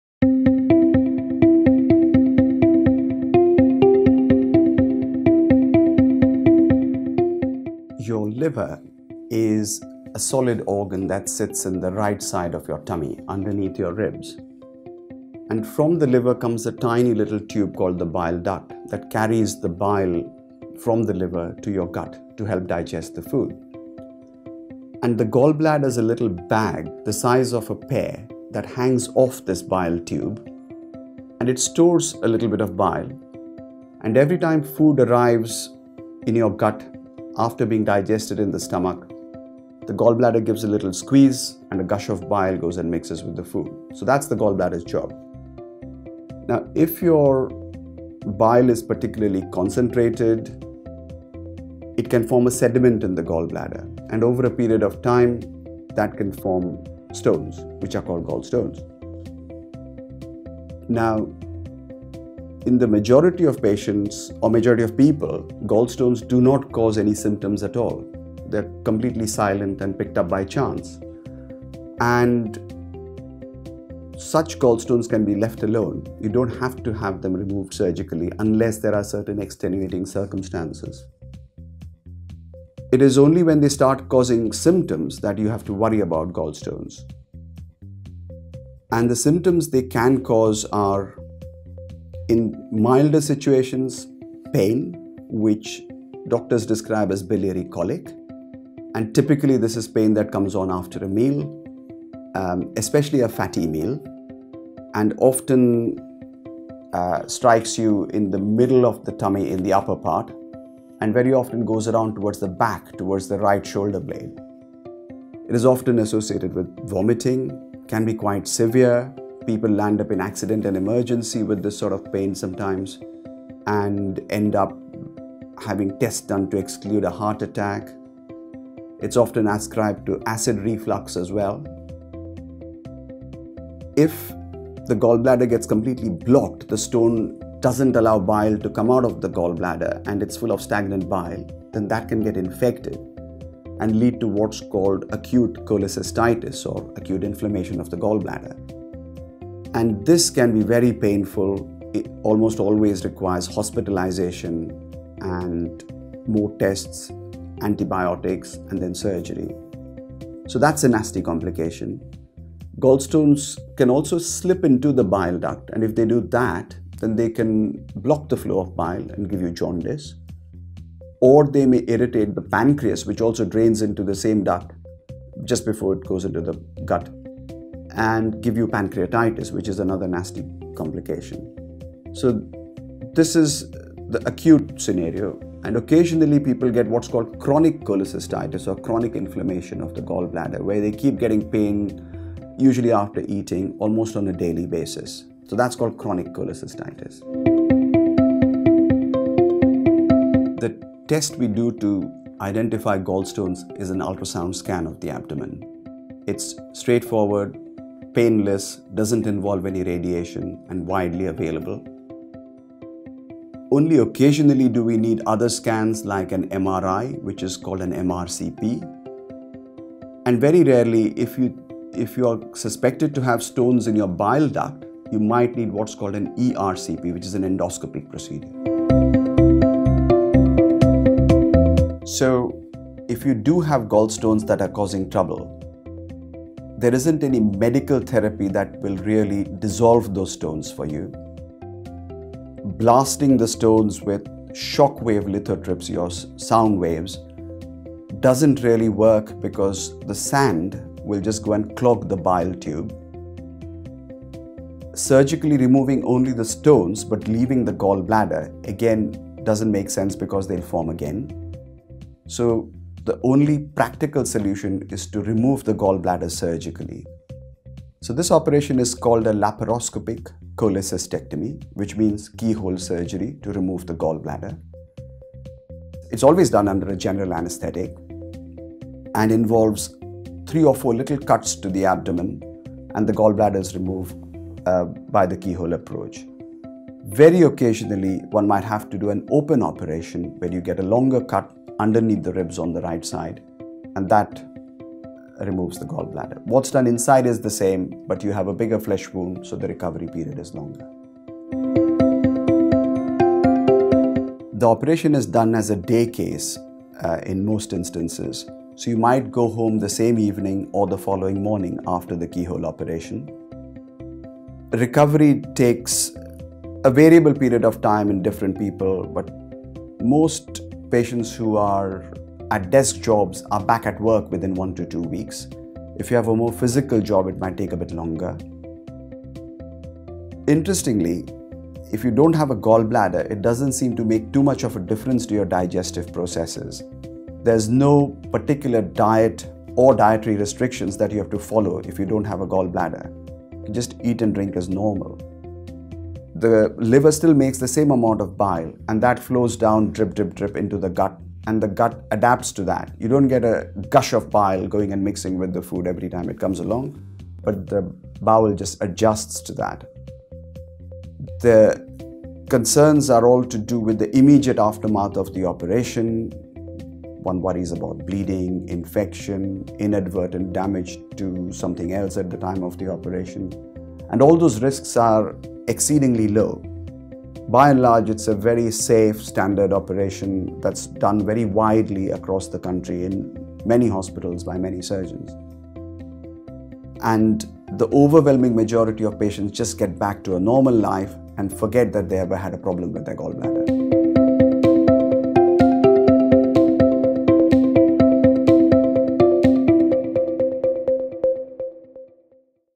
What are the symptoms of gallstones? What problems can gallstones cause?
In the majority of people, gallstones do not cause any symptoms. However, if they start to, they can cause a variety of problems. Mr Satyajit Bhattacharya, expert surgeon, explains how gallstones can cause trouble for patients, and how they can be removed.